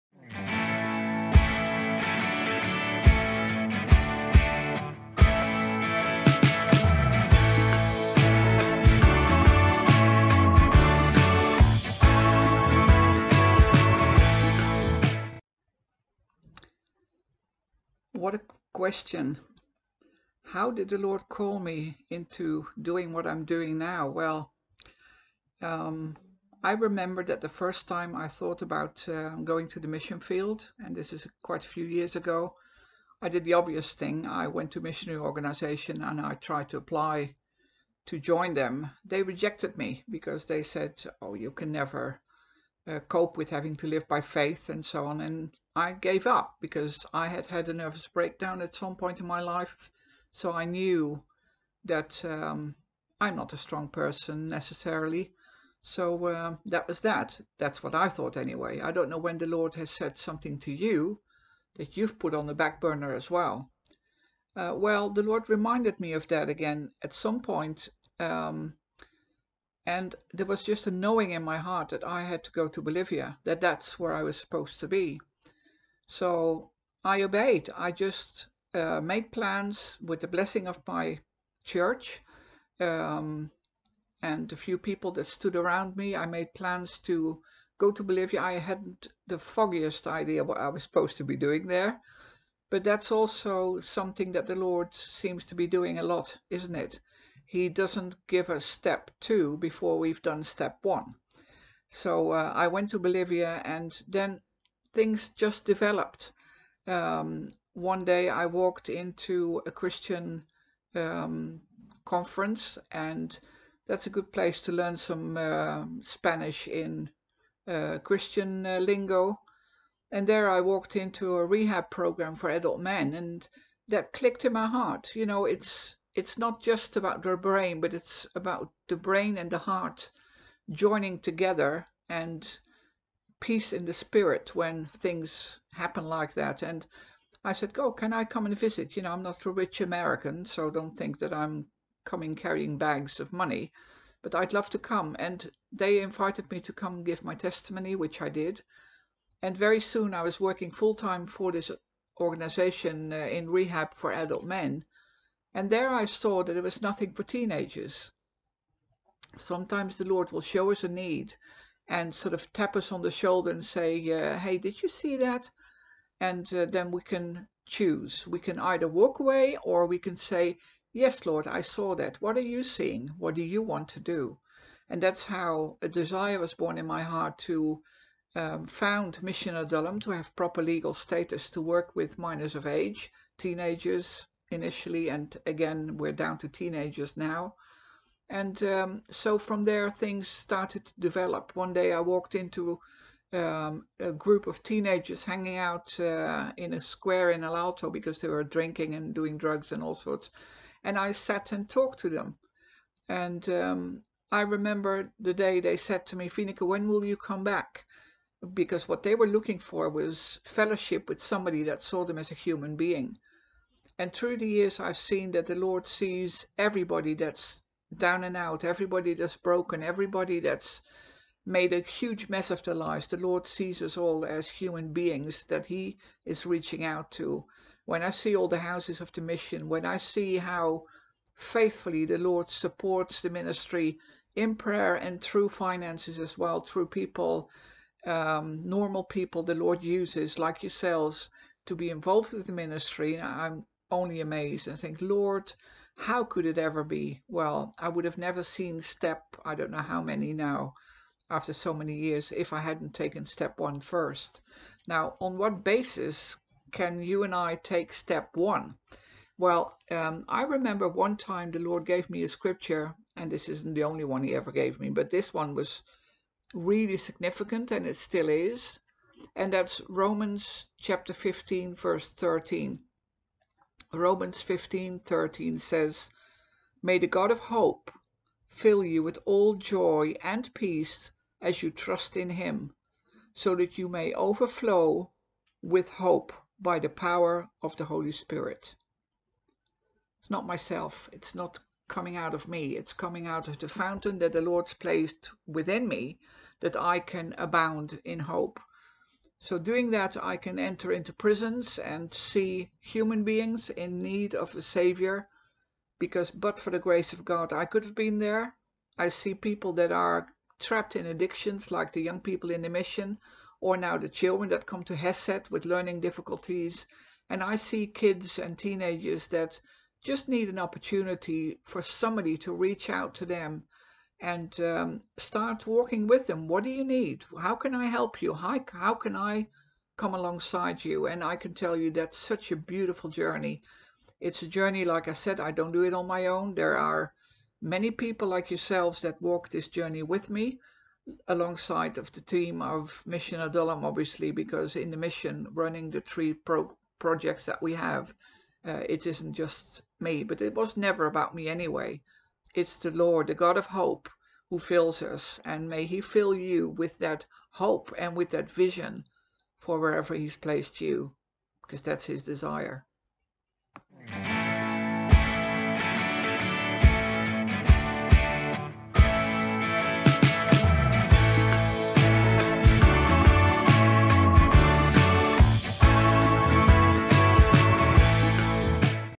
Thought for the Day with Fineke Janssen
We asked Fineke (one of the missionaries we support) to share about how God called her into mission.